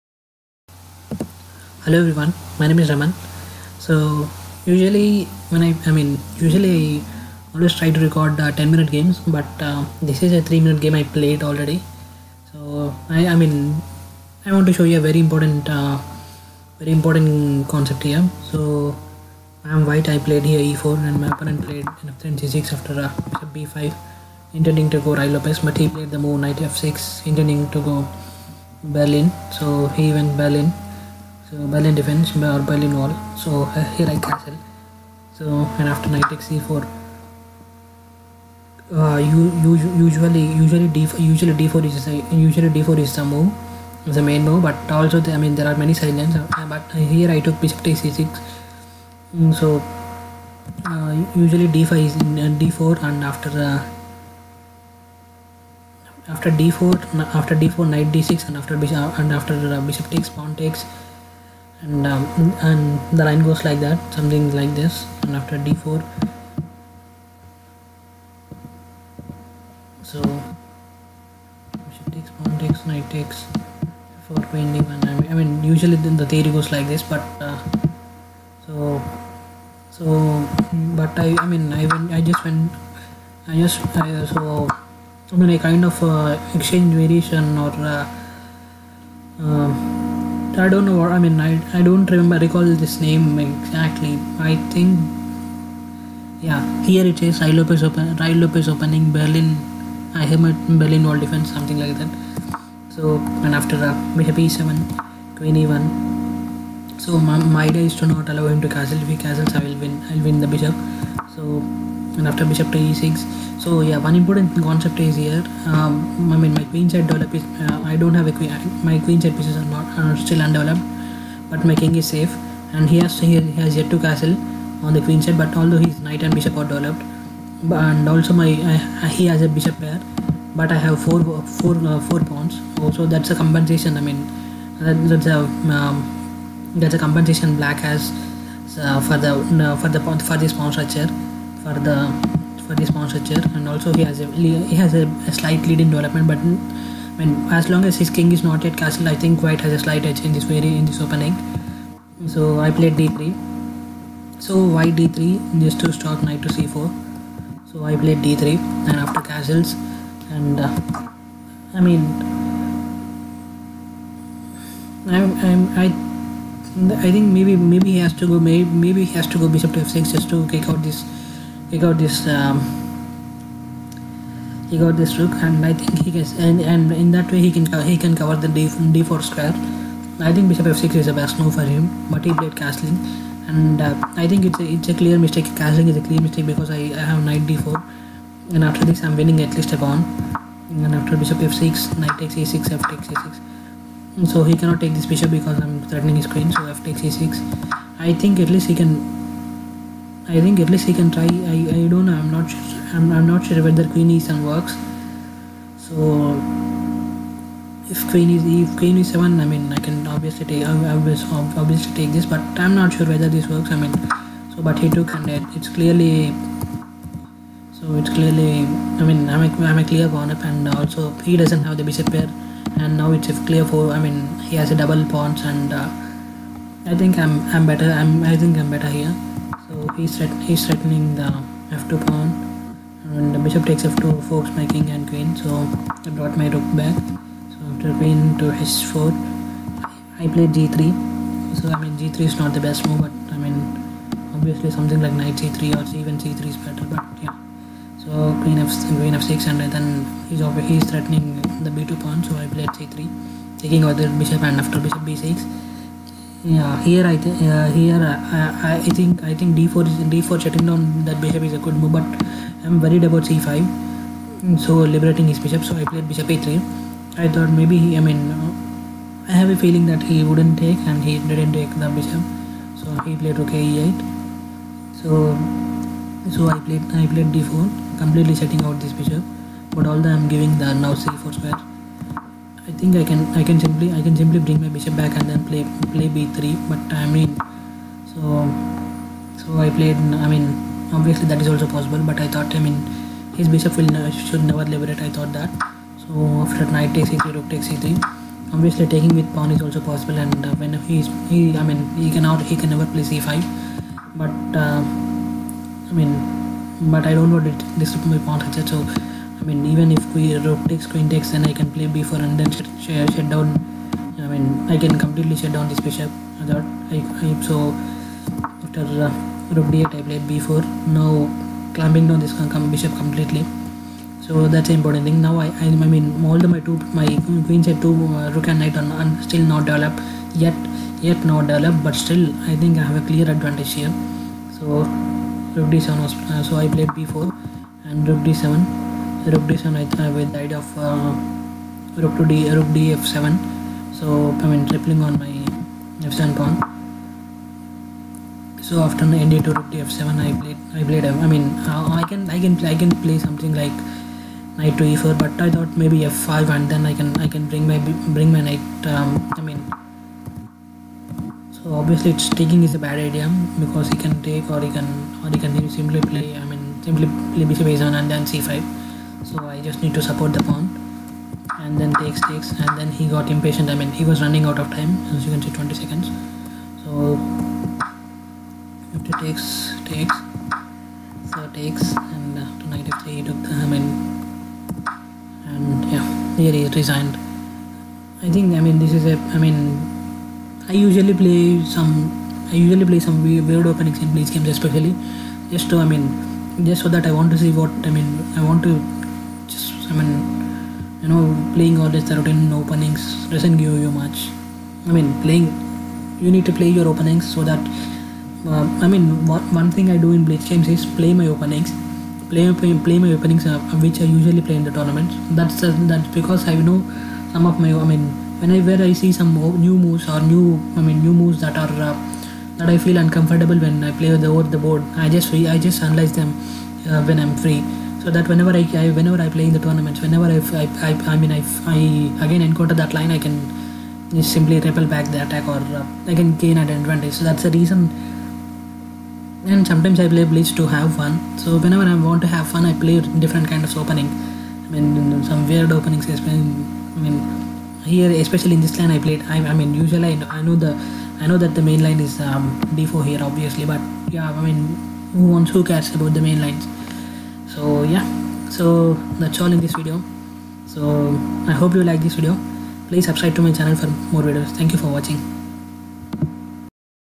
3 Min Chess Game with Some Beautiful Moves: Berlin Defence
In this video I analysed a 3 min game that I played online.